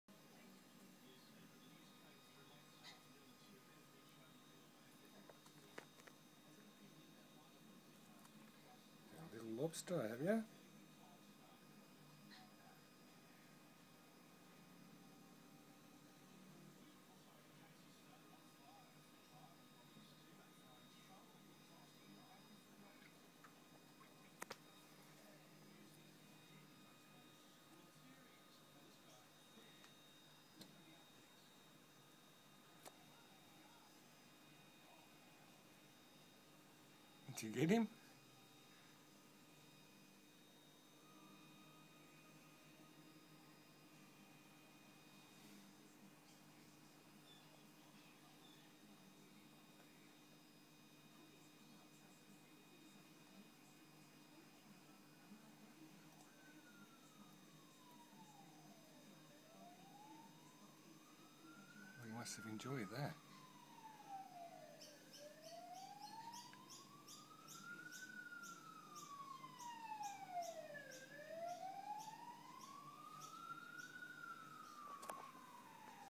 Oscar eating
Oscar having a nice feed.